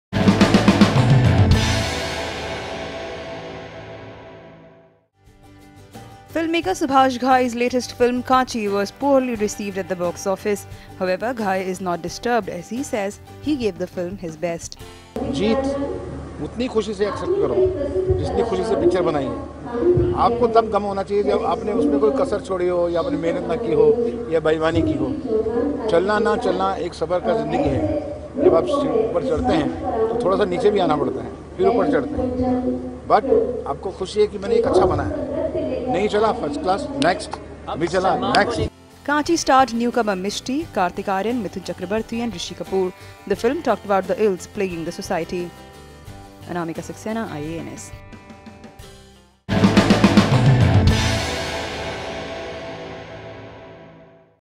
Kaanchi flop show no issue for Ghai
Filmmaker Subhash Ghai's latest film "Kaanchi" was poorly received at the box-office. However, Ghai is not disturbed as he says he gave the film his best.